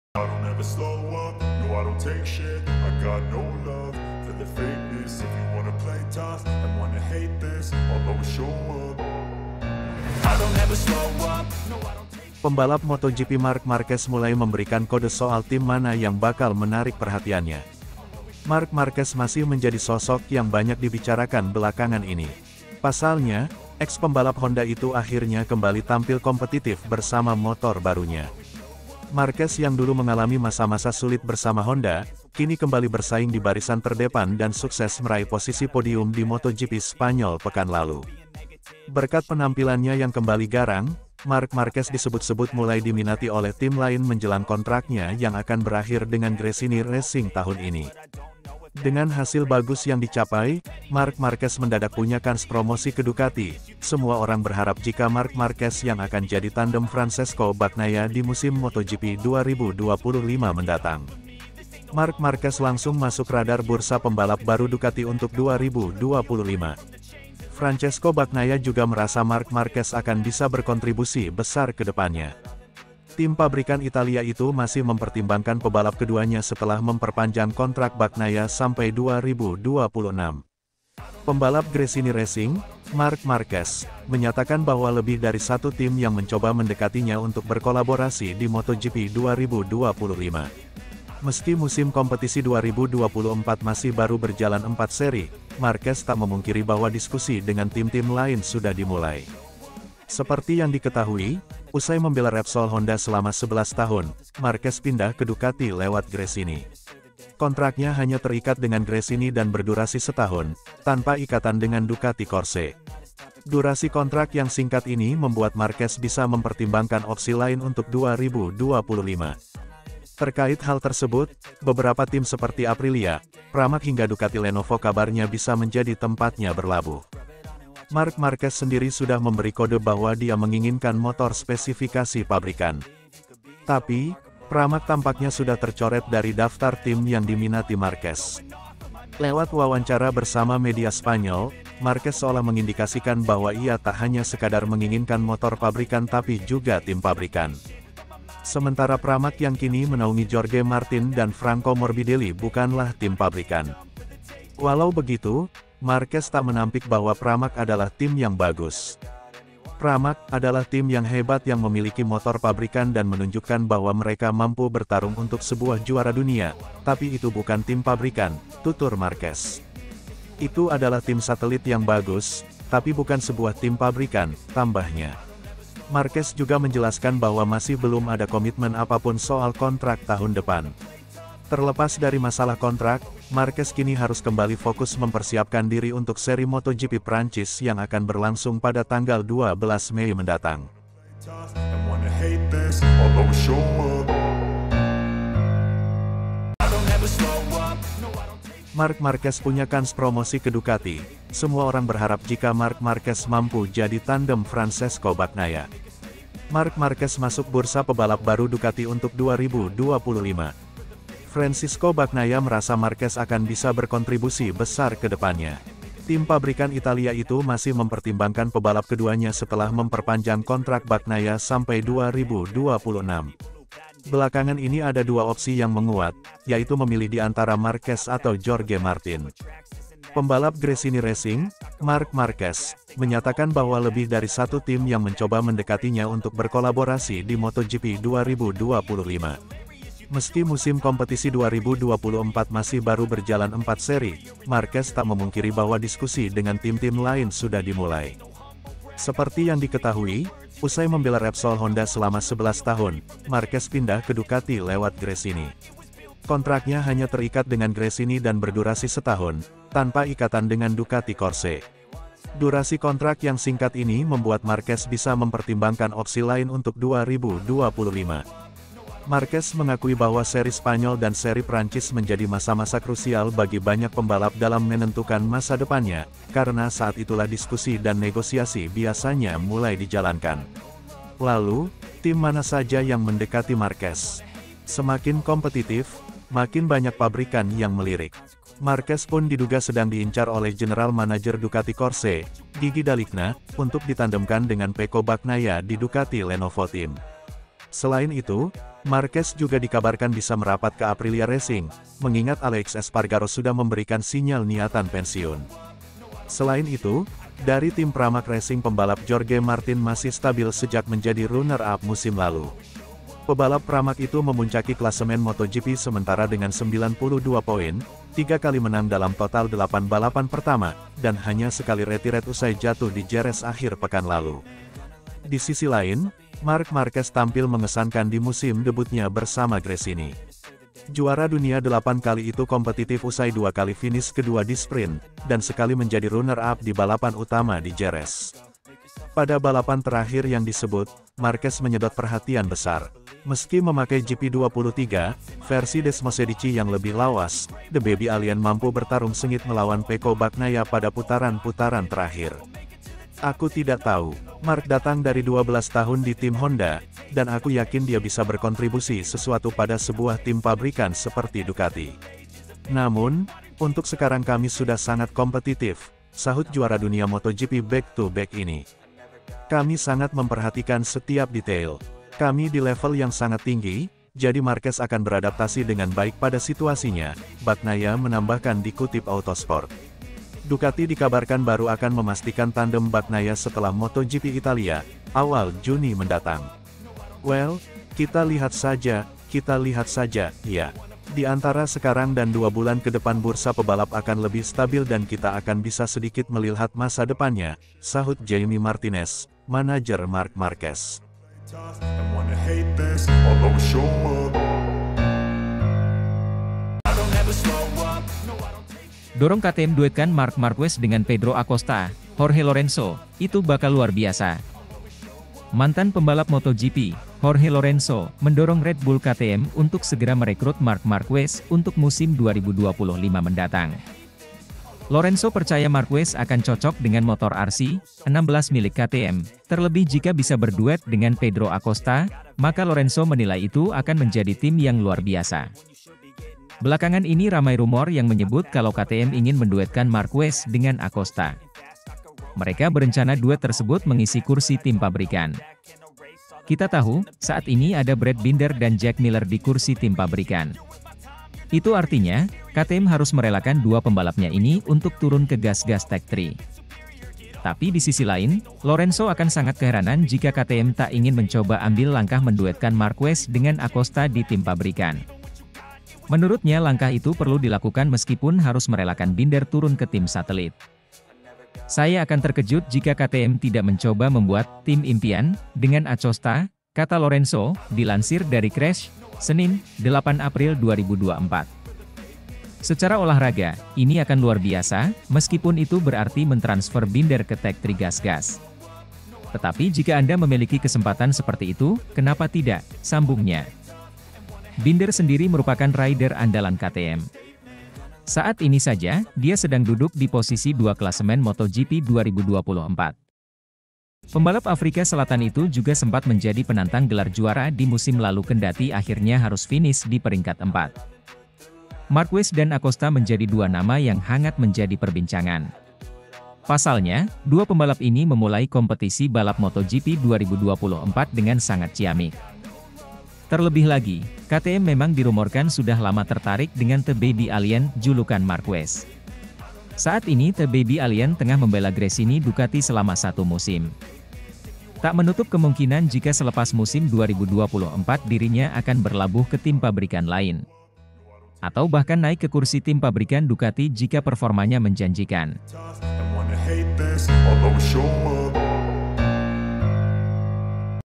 DIBONGKAR DOMENICALI🔥MARC MARQUEZ RESMI BOYONG KE PABRIKAN~GEBER DESMOSEDICI BARU~JD TANDEM BAGNAIA
DIBONGKAR DOMENICALI🔥MARC MARQUEZ RESMI BOYONG KE PABRIKAN~GEBER DESMOSEDICI BARU~JDI TANDEM BAGNAIA